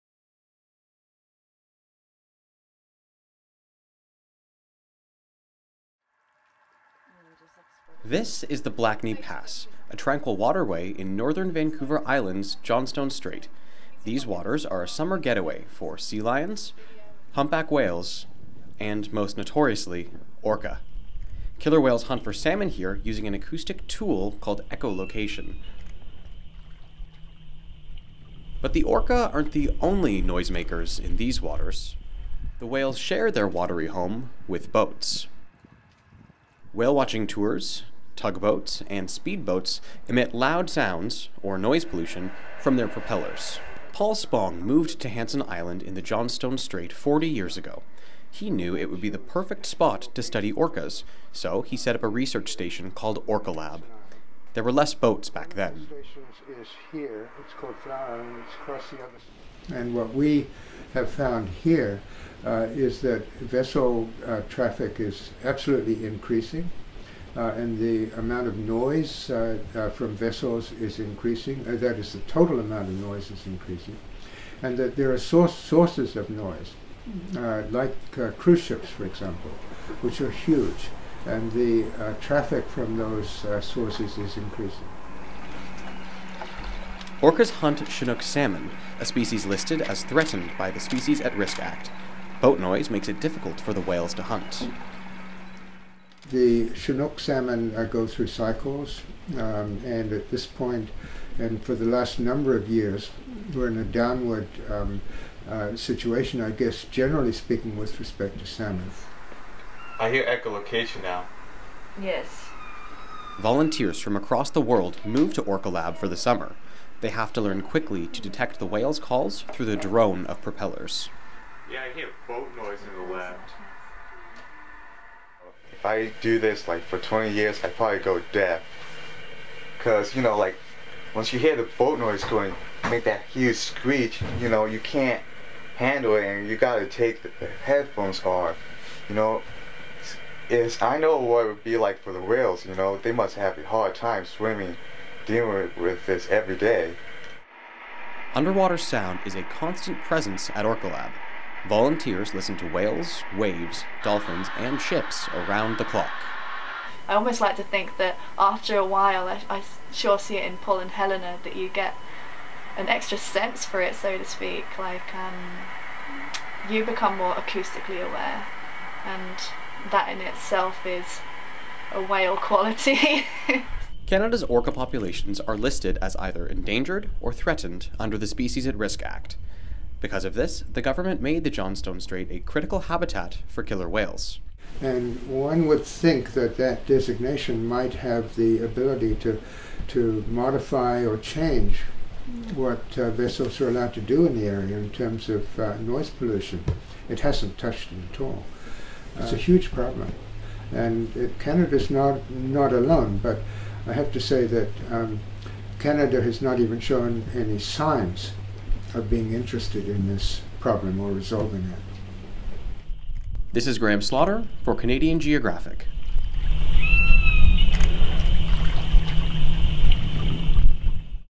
Orca Songs
Sound pollution drowns out echolocation among orcas in northern B.C. Volunteers and researchers at OrcaLab keep track of orca calls to see how shipping traffic affects them. Filmed by Marianna Angotti, produced by Graham Slaughter.